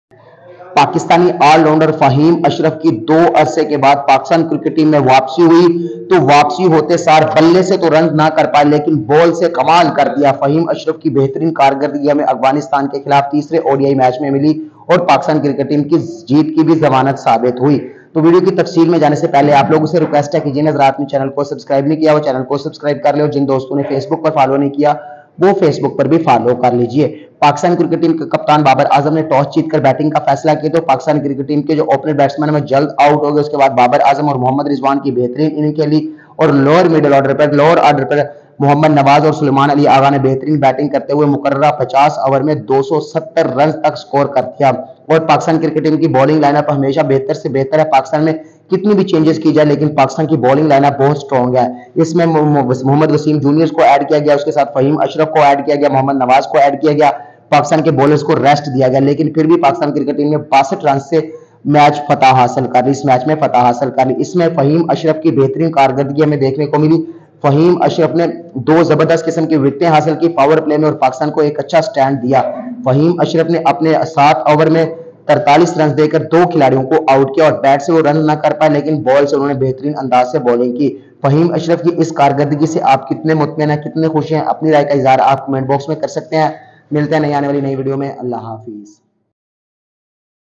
Faheem Ashraf Of Brilliant Bowling Against Afg 3rd ODI Match | Pak vs Afg Highlights 3rd ODI Match
Faheem Ashraf Of Brilliant Bowling Against Afghanistan 3rd ODI Match | Pakistan vs Afghanistan Highlights 3rd ODI Match |

Tiwtter